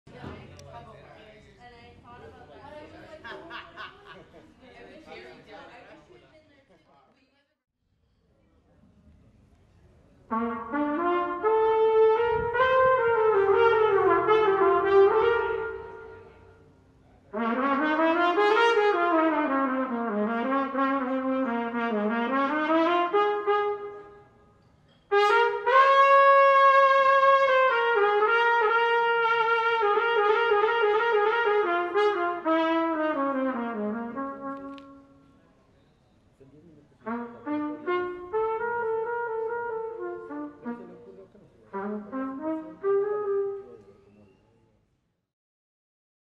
RADIO TULUM LAB SHORTS (at BOHEME) - Presenting Silver
Mr. Rock n Roll 'Silver' the great trumpeter, joined us to ponder the convergence of time ...